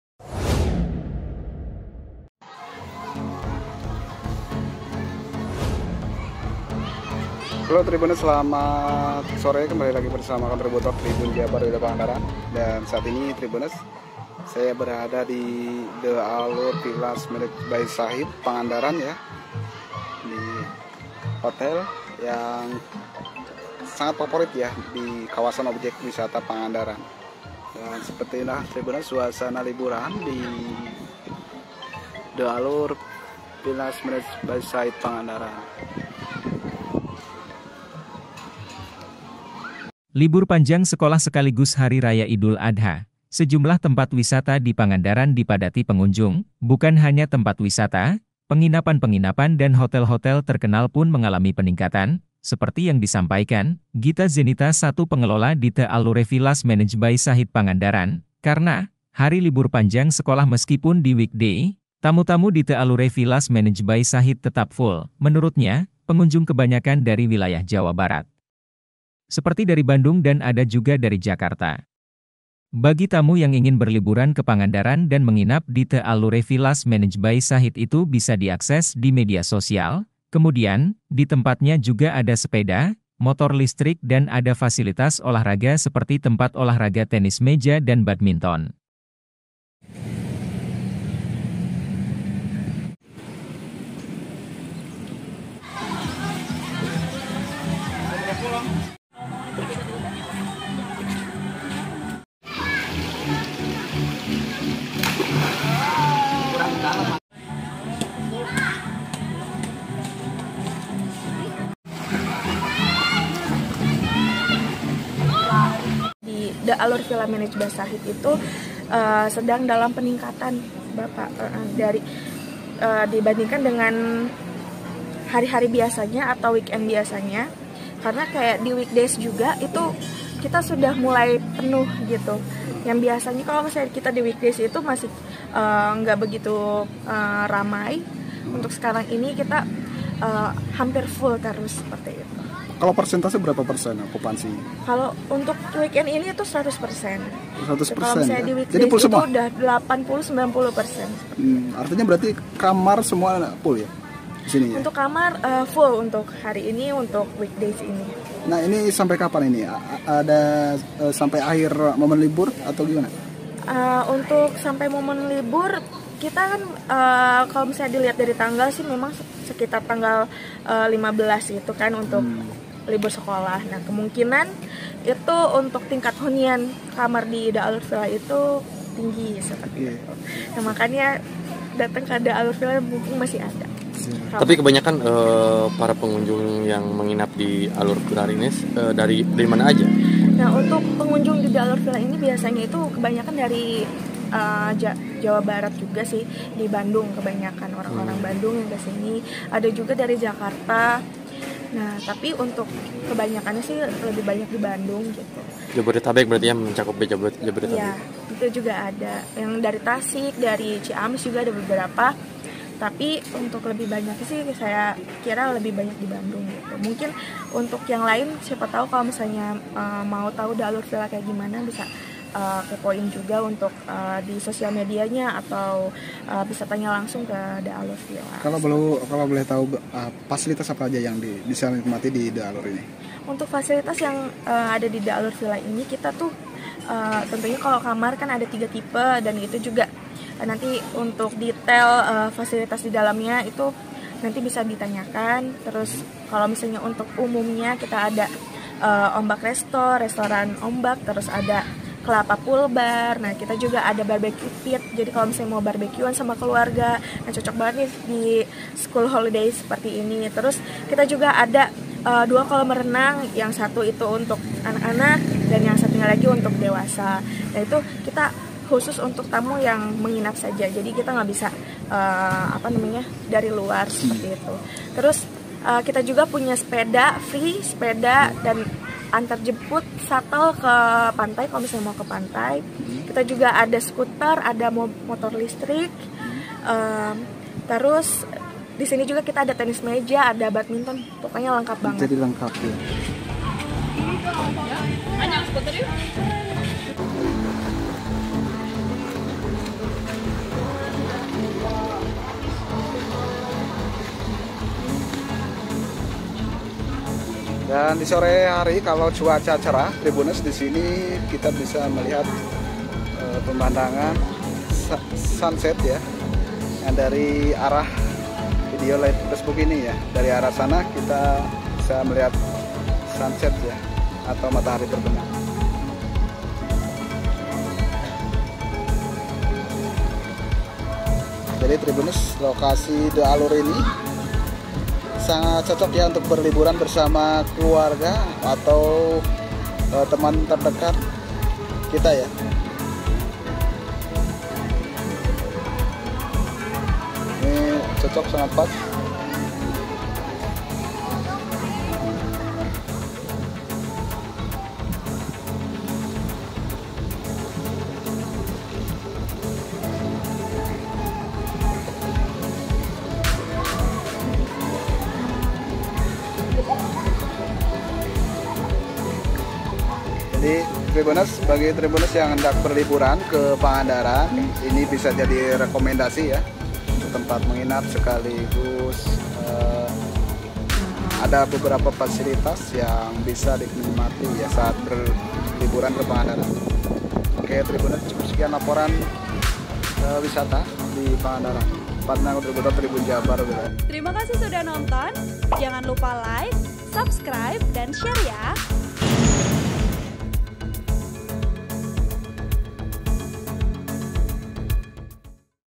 Libur Panjang Sekolah, Okupansi Hotel di Pangandaran Full., Pengunjung Kebanyakan dari Jawa Barat
Penulis: Padna | Editor: Januar Pribadi Hamel | Video Editor: Edwin Tk